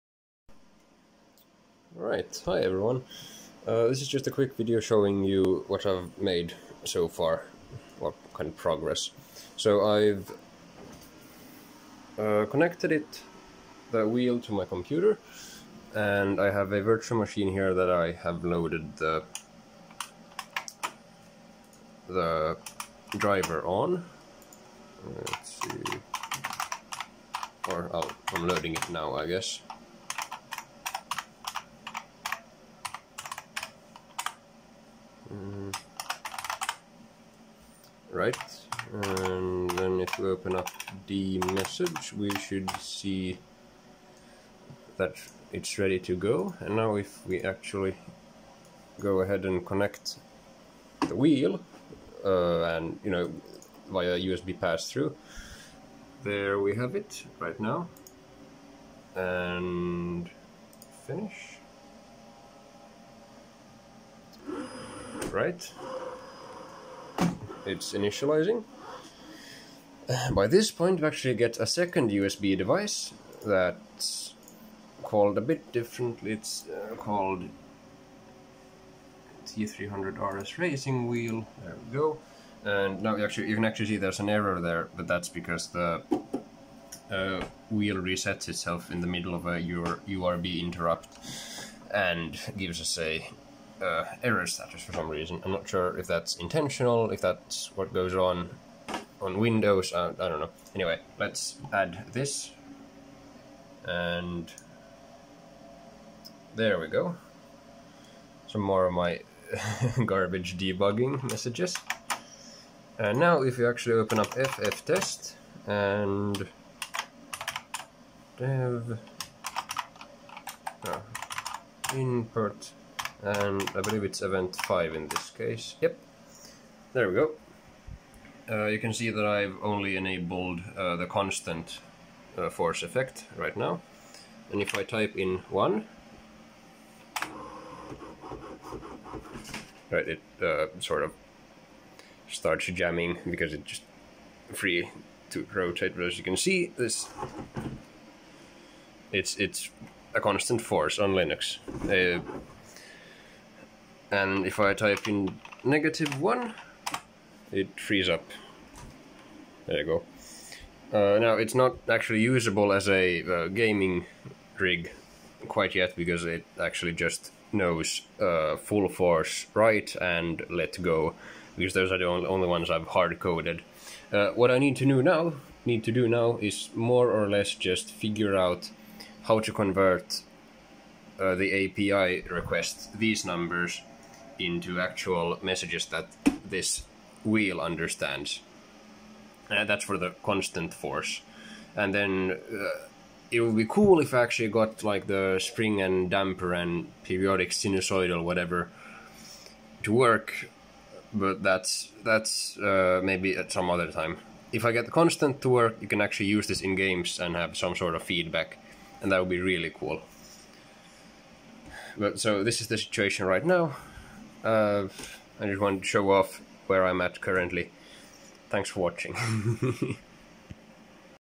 Force feedback test T300RS on Linux
First working proof of concept